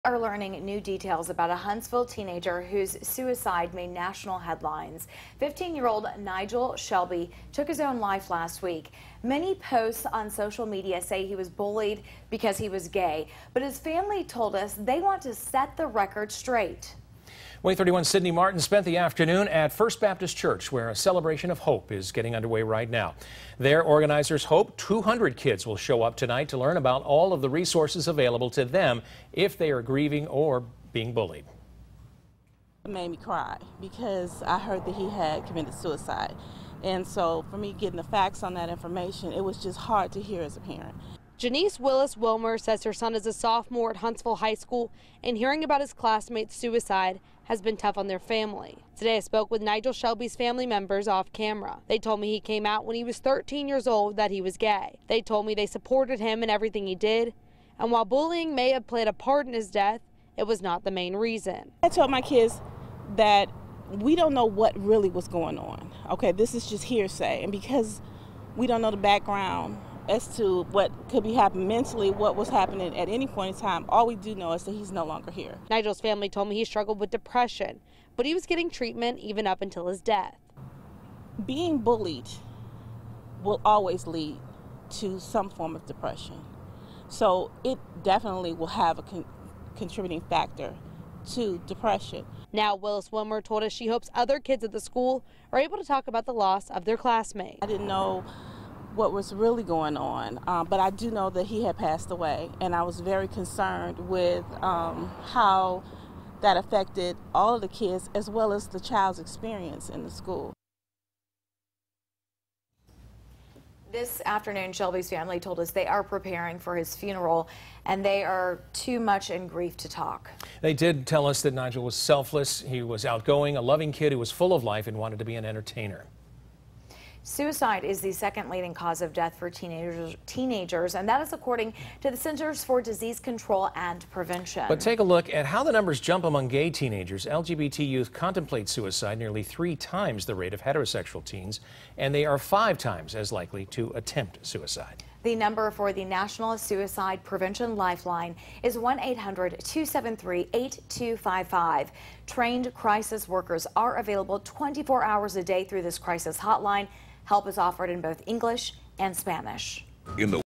Teen struggled with depression for years
We are learning new details about a Huntsville teenager whose suicide made national headlines.